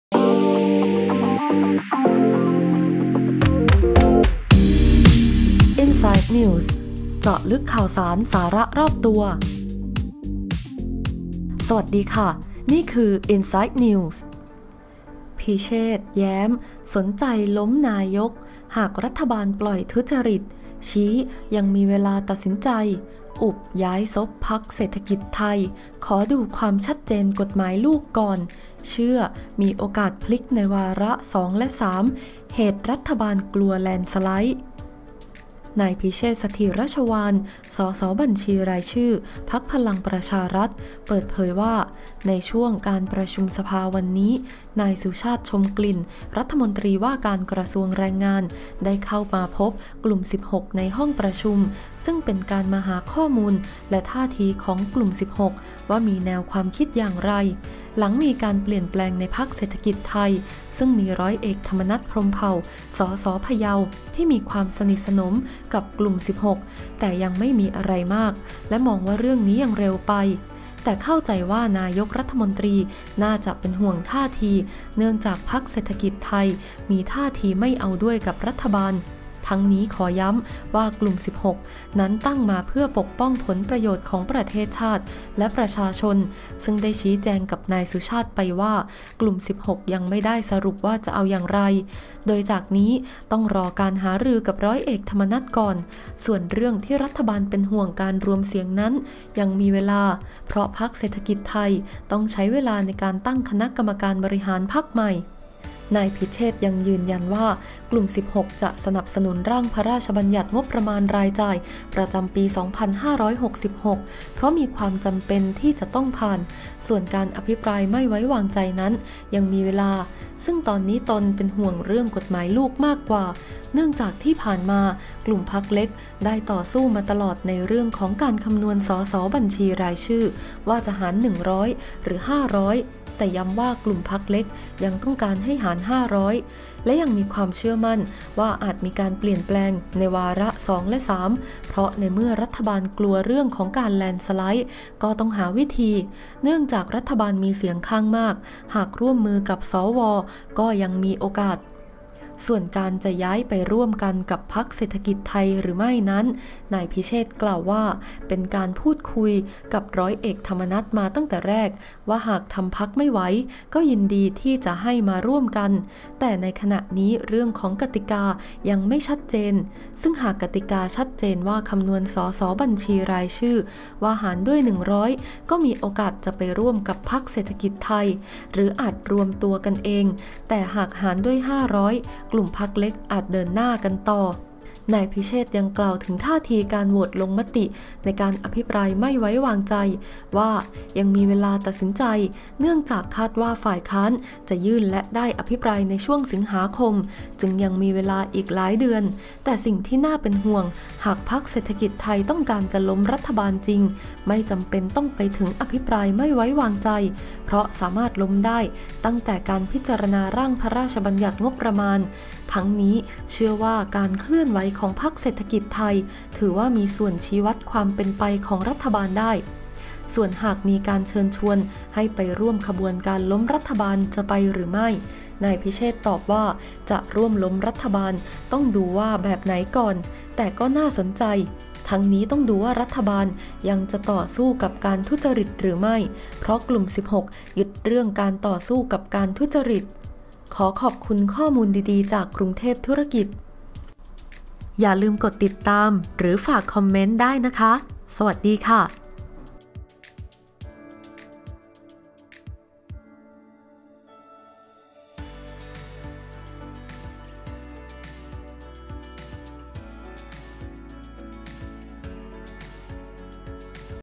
สัญญาณแรง "พิเชษฐ" รับ สนใจล้ม"รัฐบาลประยุทธ์" เผย เศรษฐกิจไทย มีส่วนชี้วัดความเป็นไป
“พิเชษฐ” แย้ม สนใจล้ม"บิ๊กตู่" หากรัฐบาลปล่อยทุจริต ชี้ ยังมีเวลาตัดสินใจ อุบ ย้ายซบพรรคเศรษฐกิจไทย ขอดูความชัดเจนกฎหมายลูกก่อน เชื่อมีโอกาสพลิกในวาระ 2-3 เหตุ รัฐบาล กลัว เพื่อไทยแลนด์สไลด์

นายพิเชษฐ สถิรชวาล ส.ส.บัญชีรายชื่อ พรรคพลังประชารัฐ เปิดเผยว่า ในช่วงการประชุมสภาวันนี้ นายสุชาติ ชมกลิ่น รัฐมนตรีว่าการกระทรวงแรง ได้เข้ามาพบกลุ่ม 16 ในห้องประชุม ซึ่งเป็นการมาหาข้อมูลและท่าทีของกลุ่ม 16 มีแนวความคิดอย่างไร หลังมีการเปลี่ยนแปลงในพรรคเศรษฐกิจไทย ซึ่งมี ร.อ.ธรรมนัส พรหมเผ่า ส.ส.พะเยา ที่มีความสนิทสนมกับกลุ่ม 16 แต่ก็ยังไม่มีอะไรมาก และมองว่าเรื่องนี้ยังเร็วไป แต่เข้าใจว่านายกฯน่าจะเป็นห่วงท่าที เนื่องจากพรรคเศรษฐกิจไทยมีท่าทีจะไม่เอาด้วยกับรัฐบาล ทั้งนี้ขอย้ำว่ากลุ่ม 16 นั้นตั้งมาเพื่อปกป้องผลประโยชน์ของประเทศชาติ และประชาชน ซึ่งก็ได้ชี้แจงกับนายสุชาติไปว่ากลุ่ม 16 ยังไม่ได้สรุปว่าจะเอาอย่างไร โดยจากนี้ต้องรอการหารือกับ ร.อ.ธรรมนัส ก่อน ส่วนเรื่องที่รัฐบาลเป็นห่วงการรวมเสียงนั้น ยังมีเวลา เพราะพรรคเศรษฐกิจไทยยังต้องใช้เวลาในการตั้งคณะกรรมการบริหารพรรคใหม่

นายพิเชษฐ ยังกล่าวถึงท่าทีการโหวตลงมติในการอภิปรายไม่ไว้วางใจ ว่า ยังมีเวลาตัดสินใจ เนื่องจากคาดว่าฝ่ายค้านจะยื่นและได้อภิปรายในช่วงสิงหาคม จึงยังมีเวลาอีกหลายเดือน แต่สิ่งที่น่าเป็นห่วงหากพรรคเศรษฐกิจไทยต้องการจะล้มรัฐบาลจริง ก็ไม่จำเป็นต้องไปถึงอภิปรายไม่ไว้วางใจ เพราะสามารถล้มได้ตั้งแต่การพิจารณาร่างพระราชบัญญัติงบประมาณราย ทั้งนี้เชื่อว่าการเคลื่อนไหวของพรรคเศรษฐกิจไทยถือว่ามีส่วนชี้วัดความเป็นไปของรัฐบาลได้